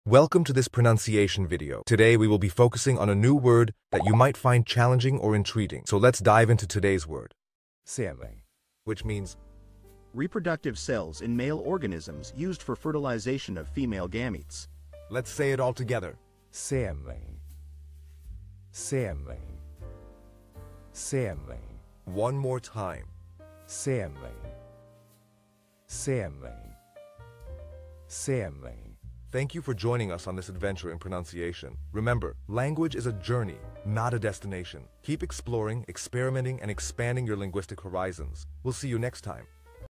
How to pronounce Sémen
Master the Pronunciation of 'Sémen Which Means Sémen' - which means : Reproductive Cells in Male Organisms Used for Fertilization of Female Gametes. 📝 with @PortuguesePronunciation 🎓🗣️ - [Your Guide to Portuguese]

Learn to pronounce 'Sémen' perfectly in Portuguese with our easy and engaging tutorial by @PortuguesePronunciation! 📖📽️ In this comprehensive video, we provide you with native speaker examples 🗣️👂 and phonetic breakdowns 🎵 to ensure you get the pronunciation just right.

How to say Semen in Portuguese